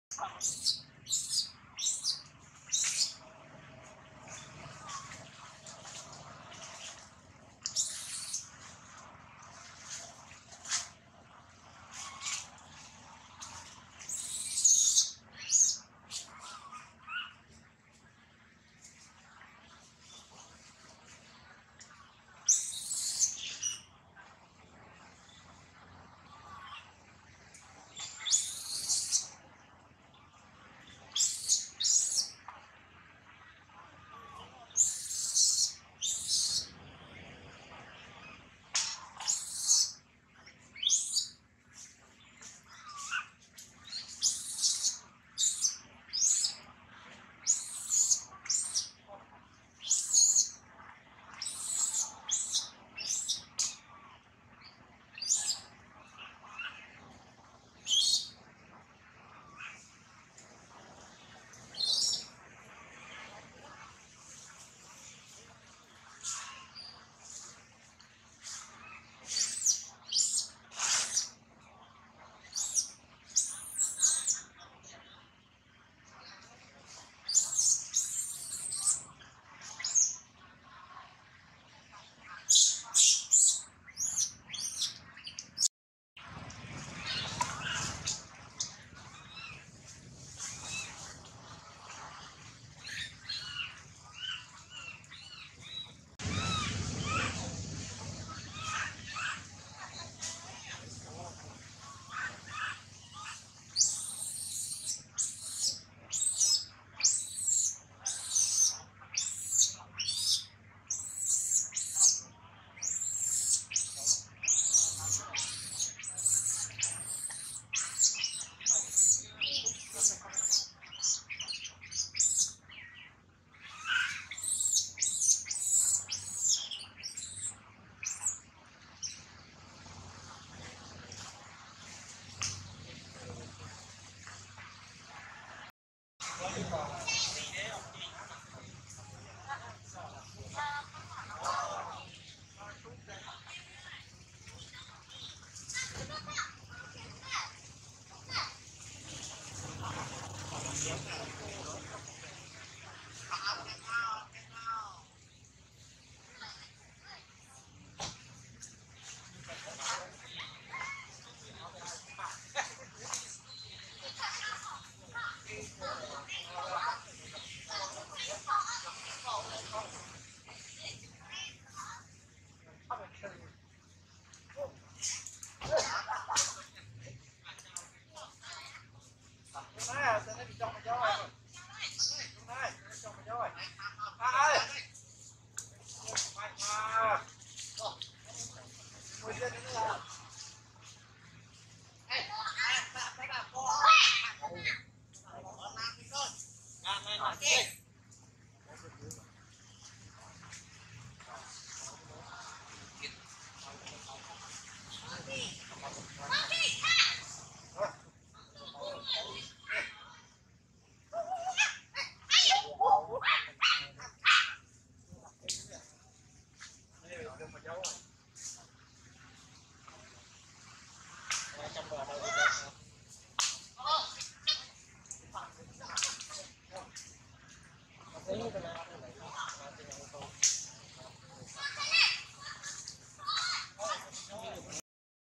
Baby Lori cry seriously cos scare of fall down, baby much crying.
mommy does no care baby monkey and let her daughter cry very loudly on the branch.